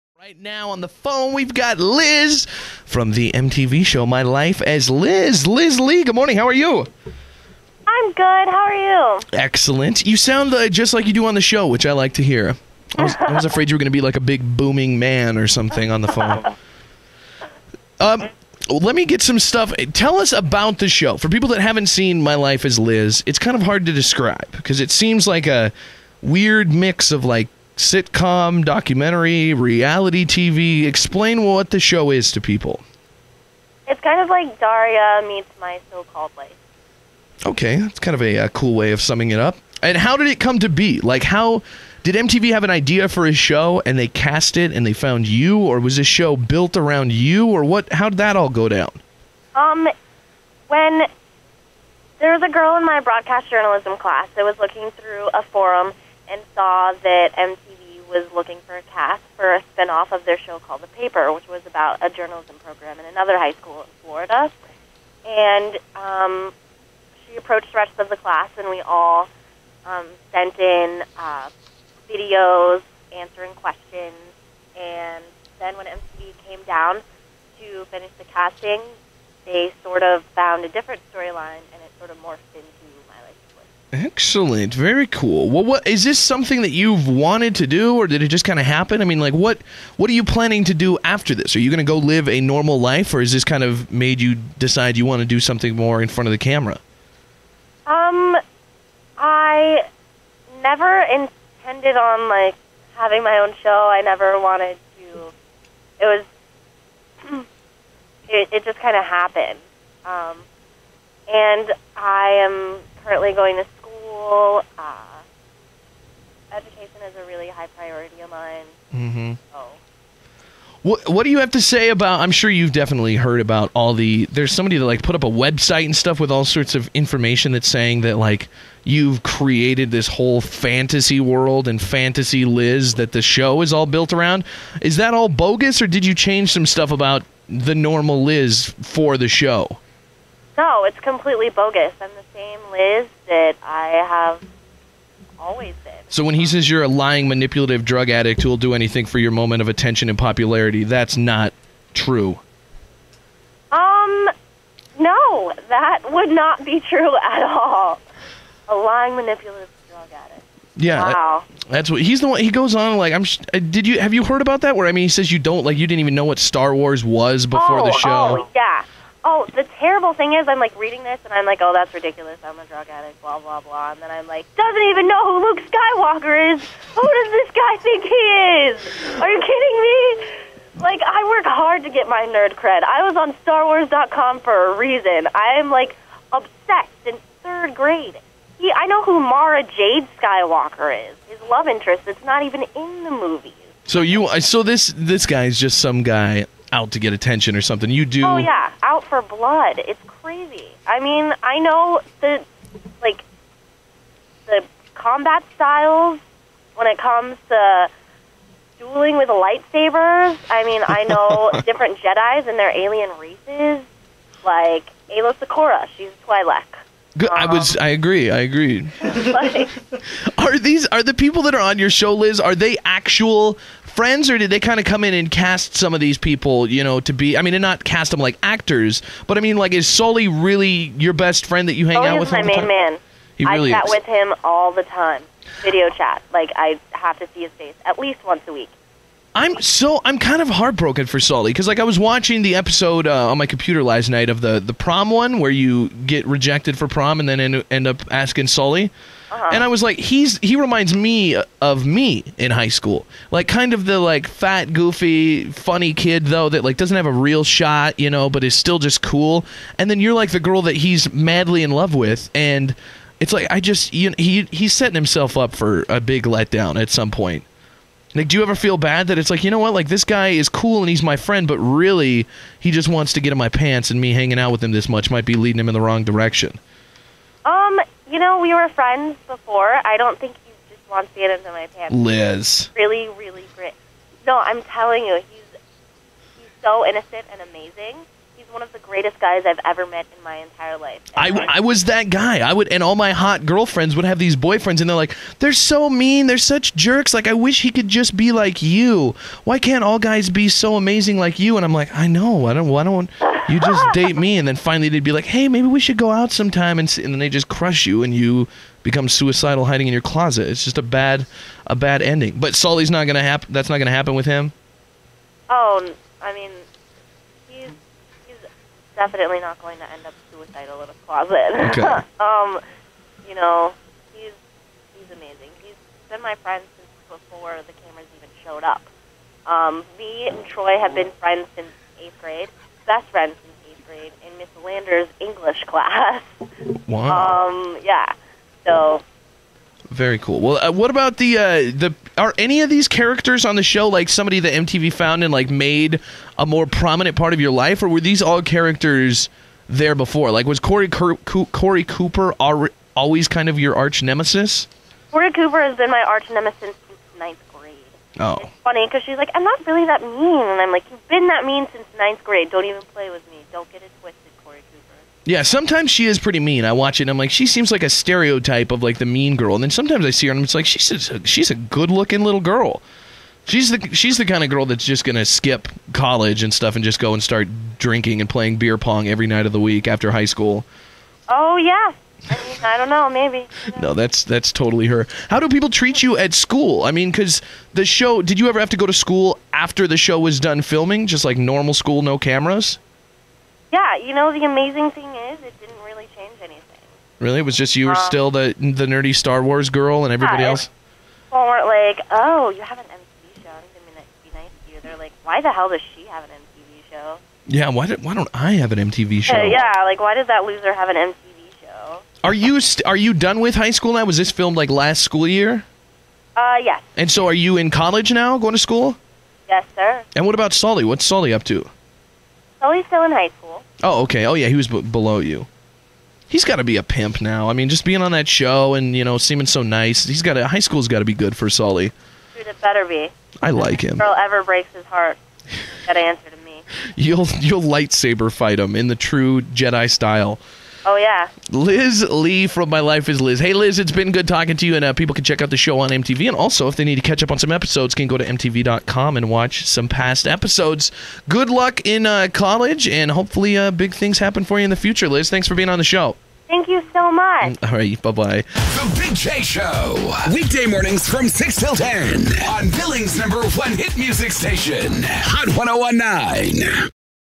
My Life as Liz - Liz Lee Interview
Liz Lee, star of MTV's "Life as Liz", calls Big J to talk about the show and whether what and who she claims on the show is, in fact, true.